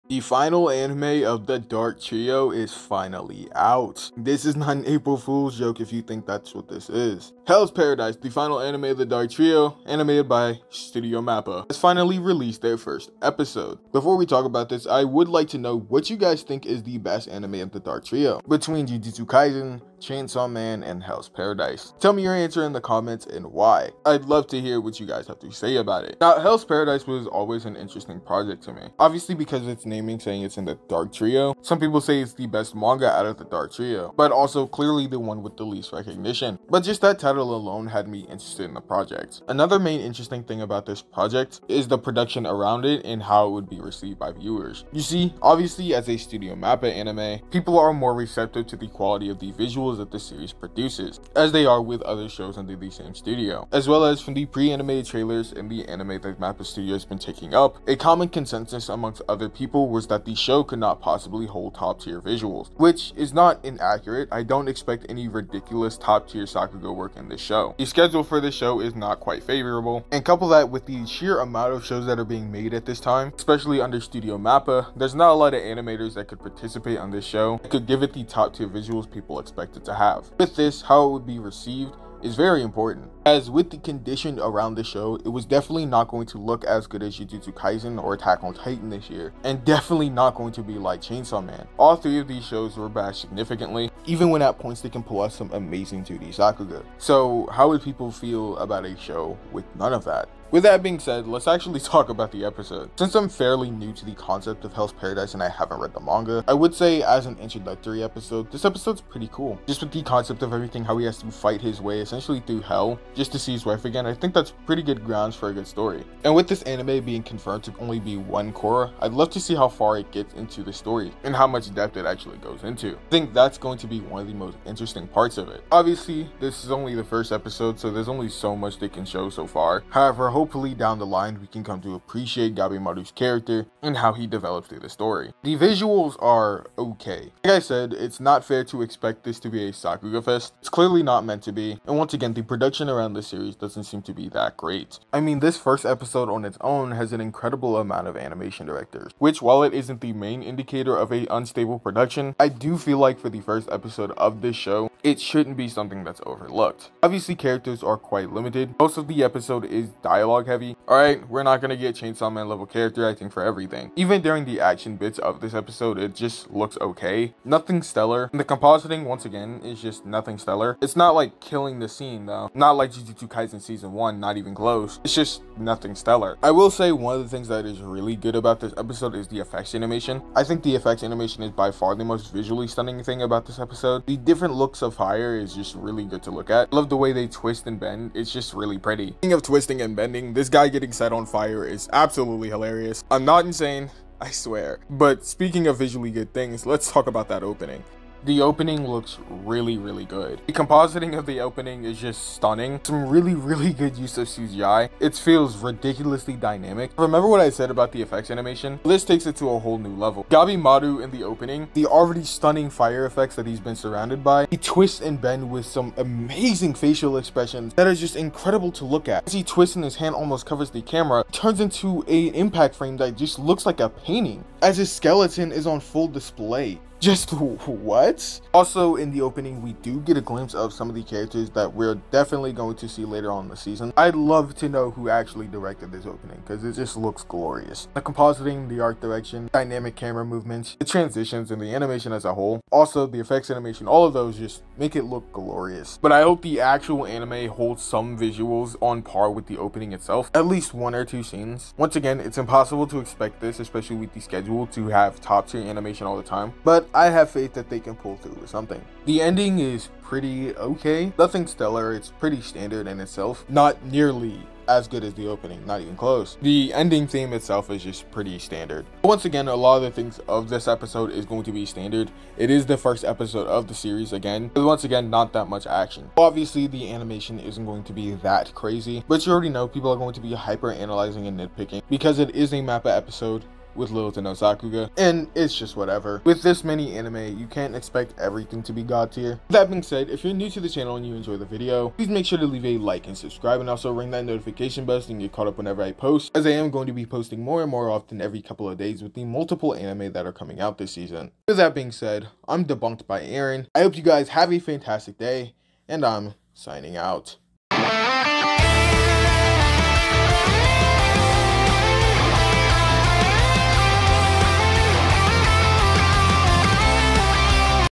The Dark Trio is Complete!!
the first episode of hells paradise has finally come out. it is also the final show of the dark trio under studio mappa. though not sharing the best animation of mappa shows,some of the effects animation looks absolutely stunning and I hope it continues through the rest of the series. the show doesn't have the best production or schedule,but I think mappa got this in the bag.

music in vid: white bat audio